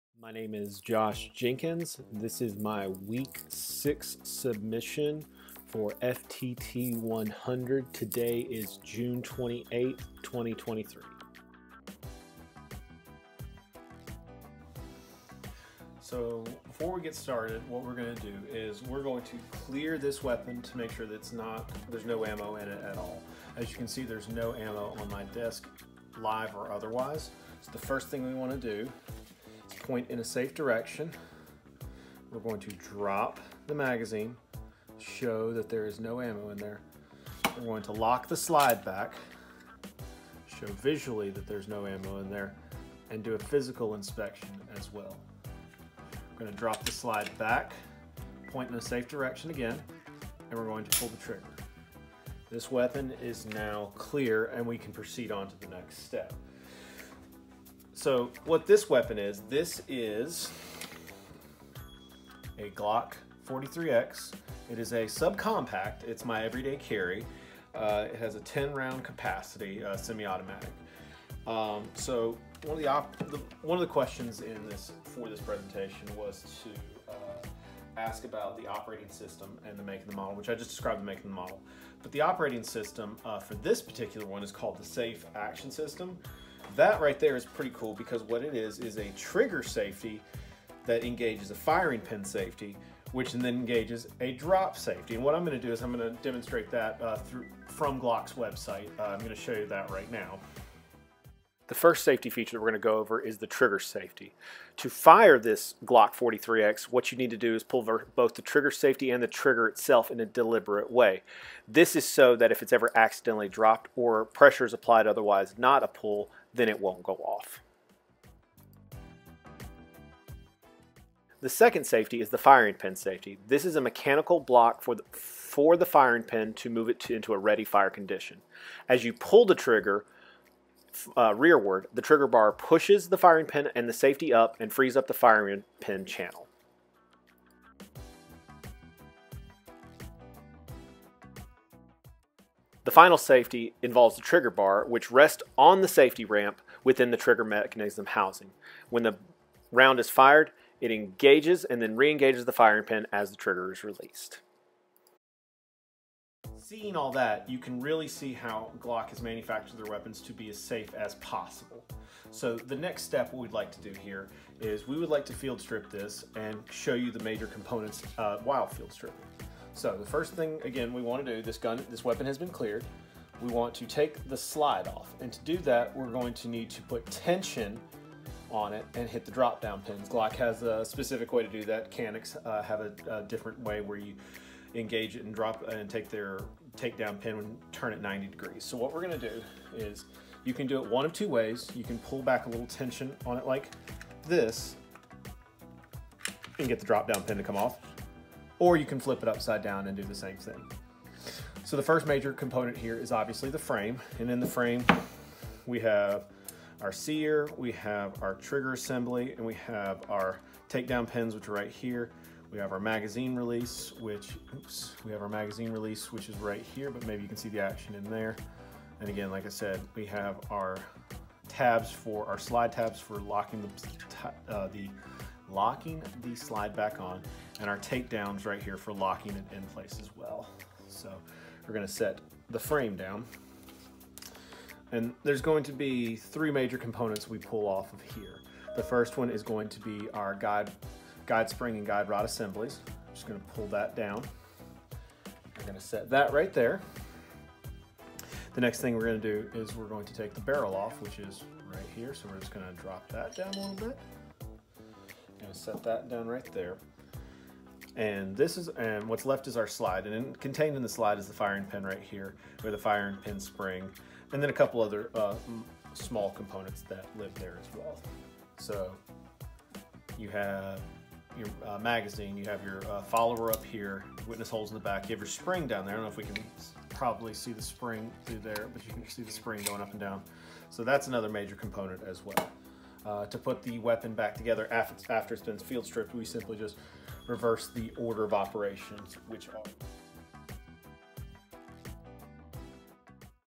FTT 100 - Week 6 - Assignment
IMPORTANT NOTE: For this assignment, it is either a requirement or you may choose to disassemble, reassemble, and/or manipulate your firearm/firearm components. It will be important that you follow all firearm safety rules as well as safely remove all live ammunition from your firearm and your workspace (if live ammunition is present) prior to recording your video. If you fail to do this, it will result in you receiving a failing grade for the assignment, no matter how well you do at the other assignment requirements.

For this assignment, we will require you to provide either a video or photographic documentation recording your processes.

Choose an operational firearm which you have legal access to in order to perform your work this week.

Describe this firearm's operating system, the make, and the model.
Relying on the guidance and resources that you have received in this course, find a schematic of your firearm.

Properly field strip your firearm and document the process. In addition to the resources and guidance you received in this course, you can also search outside resources to find additional guidance in the field stripping process if needed. If additional resources were needed, it will be important that you document this and properly cite these resources.

During the field stripping process, document and name the major subassemblies that are separated. SDI Gunsmith @sdiedu